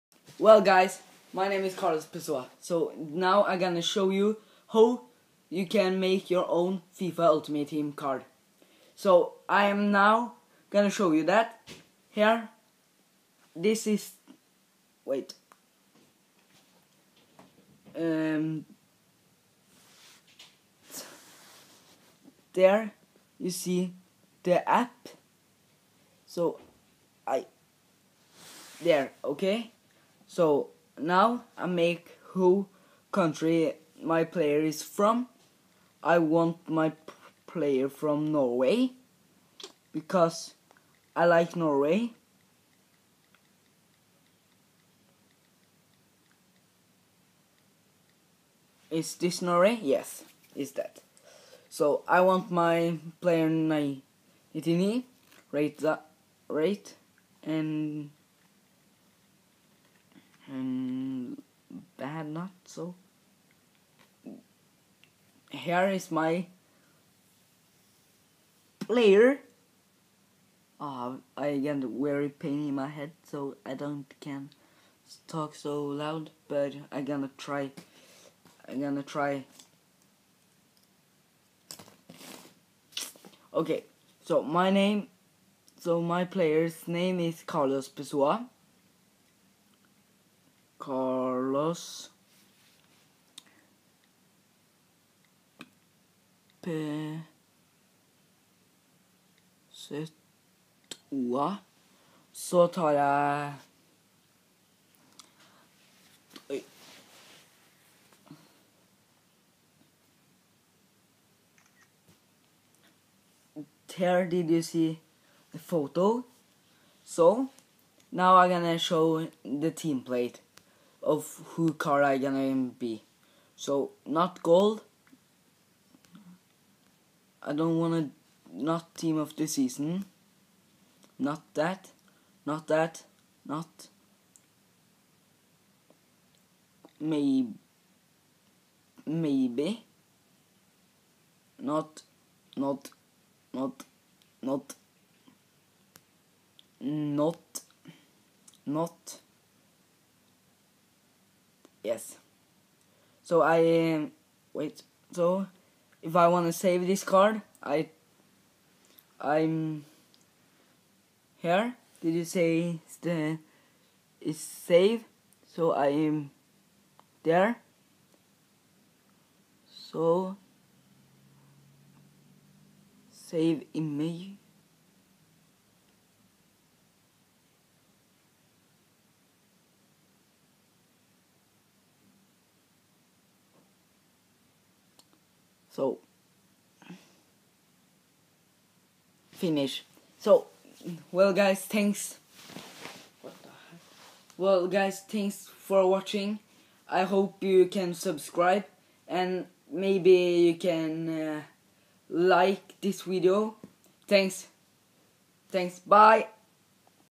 How to make your own FIFA ultimate team card
I show you how you can make your own FIFA card👍👍👍